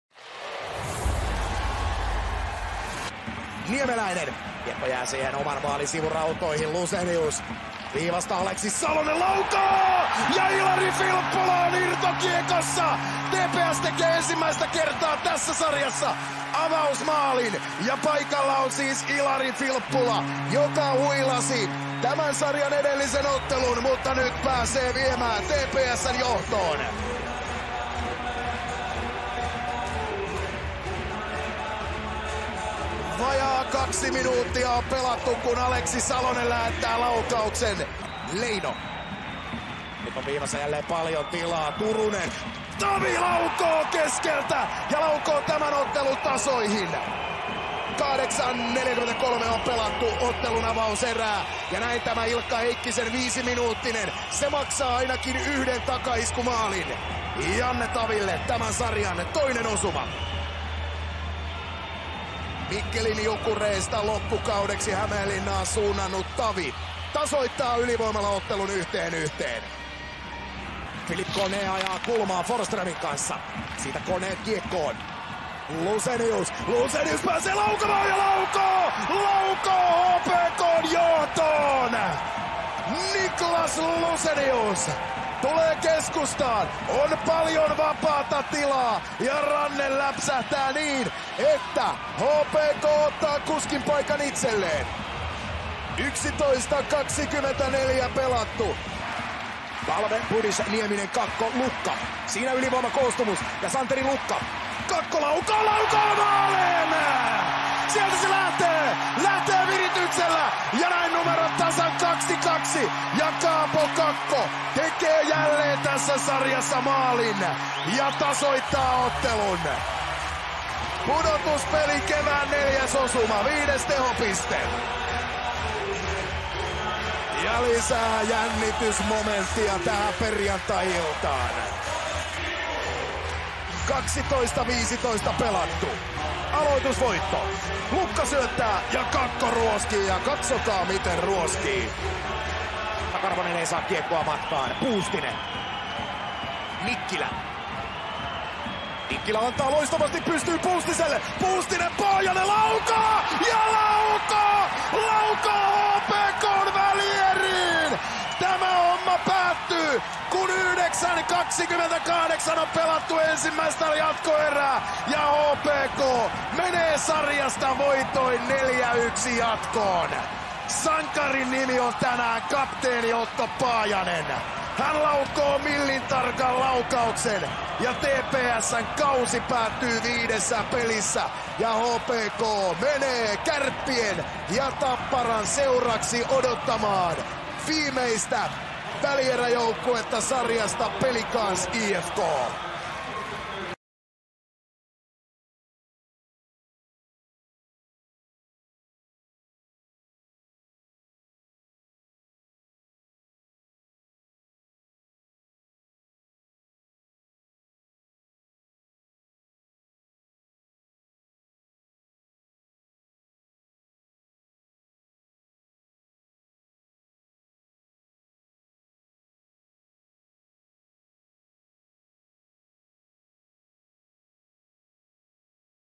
TPS – HPK 5. puolivälierä 29.3.19
HPK pelasi itsensä komeasti mitalipeleihin! TPS:n kausi päättyi pettymykseen.

TPS - HPK 2 - 3 JE. (1 - 1, 0 - 1, 1 - 0, 0 - 1)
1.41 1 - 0 Ilari Filppula (Otto Nieminen, Aleksi Salonen)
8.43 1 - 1 Janne Tavi (Teemu Turunen, Robert Leino) YV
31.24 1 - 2 Niclas Lucenius (Philippe Cornet, Eetu Tuulola)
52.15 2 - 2 Kaapo Kakko (Santeri Lukka, Oula Palve) YV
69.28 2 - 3 Otto Paajanen (Valtteri Puustinen, Petteri Nikkilä)
Henrik Haukeland, TPS 27 torjuntaa
Emil Larmi, HPK 39 torjuntaa